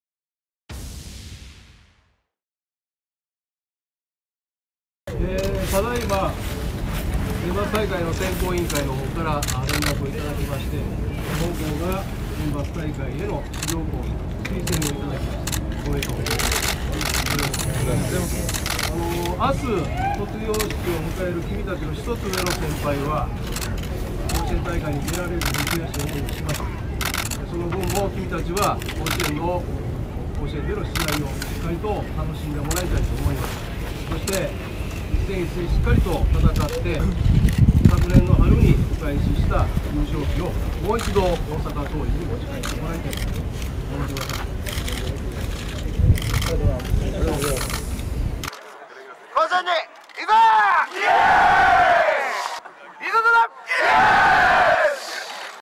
大阪桐蔭センバツ出場が決定 【日刊スポーツ】
第92回選抜高校野球大会（3月19日から13日間、甲子園）の出場32校を決める選考委員会が24日、大阪市内で開かれた。午後3時に21世紀枠、続いて一般選考の出場校が発表された。